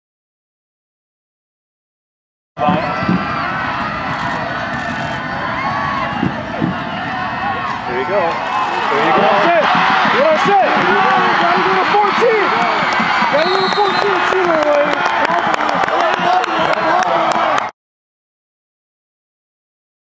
Trevor Wright (Upland,CA) Catch and Score vs Santi
Trevor Wright (Upland,CA) Catch and Score vs Santiago - CIFSS Playoffs 2013